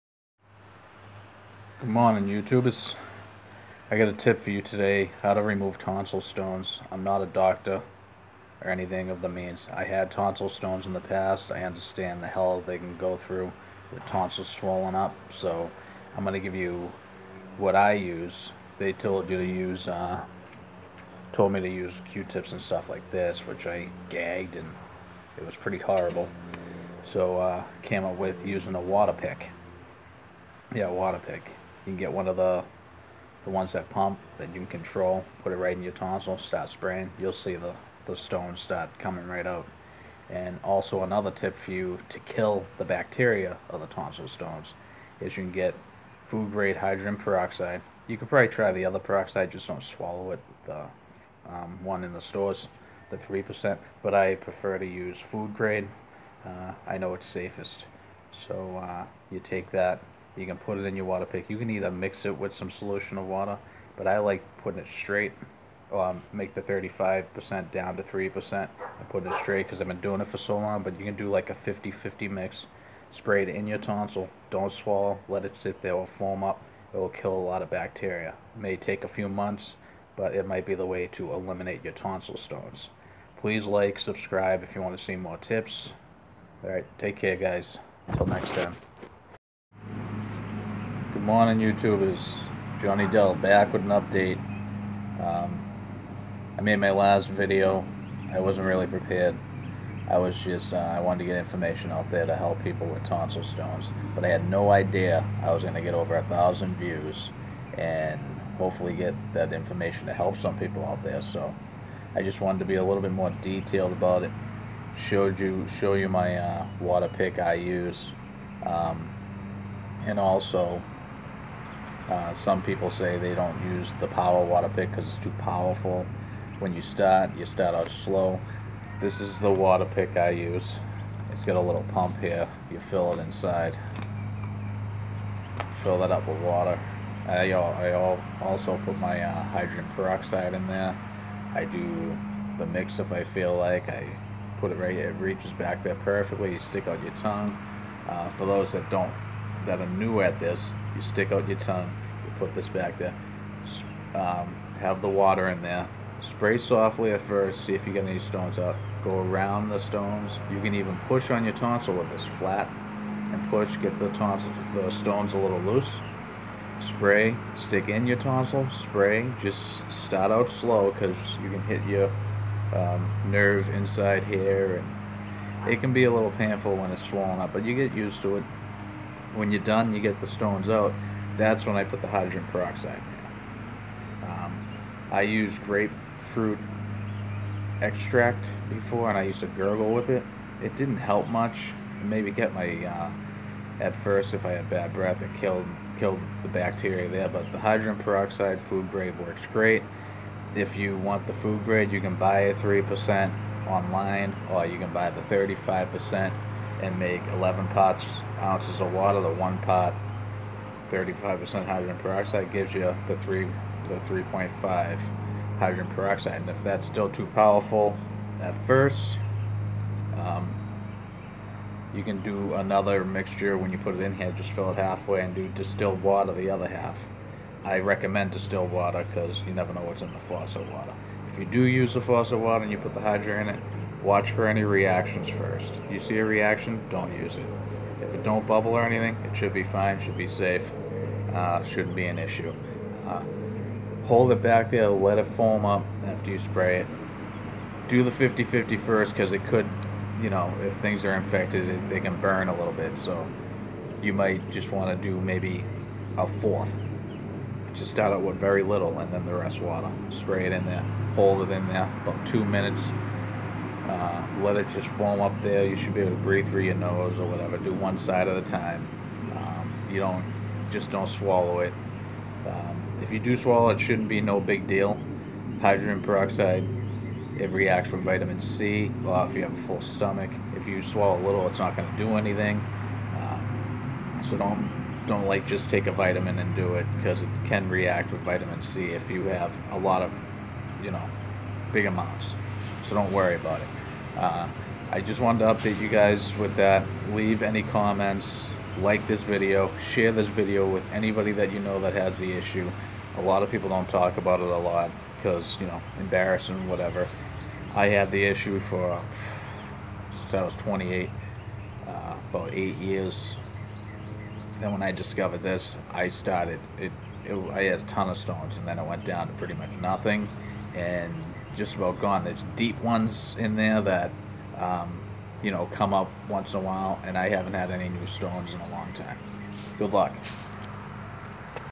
Remove tonsil stones
What i use to remove tonsil stones. I advised  the information is for my own personal use only its not ment to prevent or cure anything.